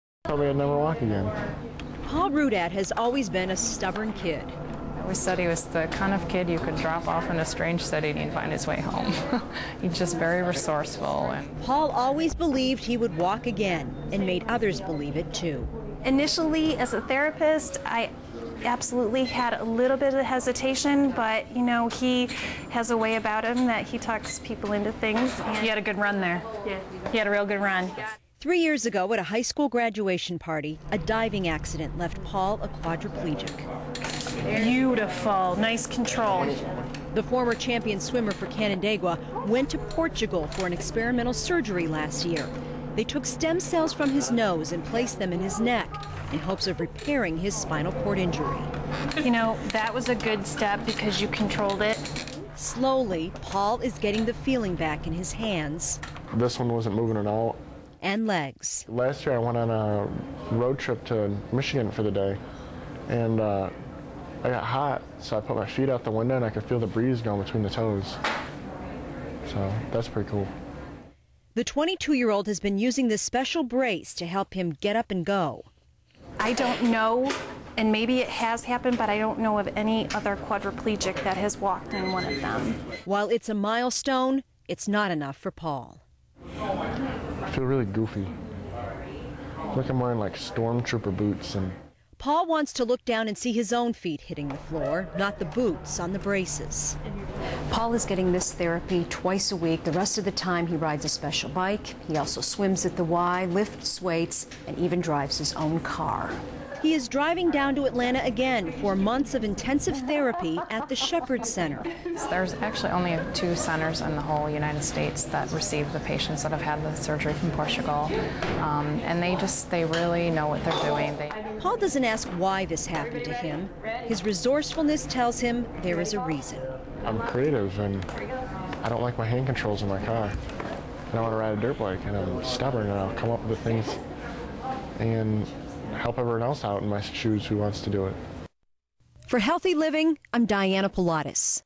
Quadriplegic walks after own cell transplant
Another of Dr. Lima's patients, Paul Rudat walks with braces after a stem cell transplant and a lot of hard work and determination.  Full story